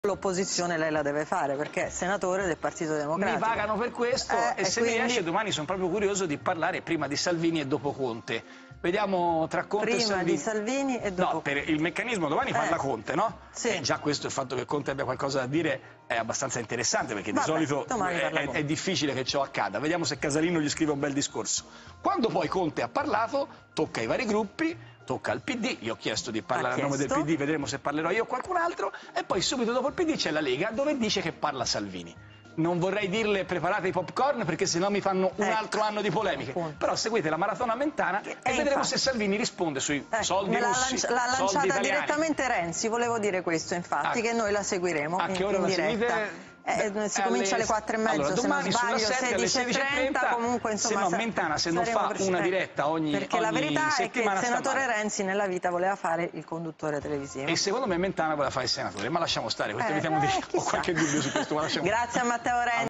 Renzi: 'Domani parla Conte vediamo se Casalino gli scrive un bel discorso'
Matteo Renzi domani alla seduta del Senato ho chiesto di parlare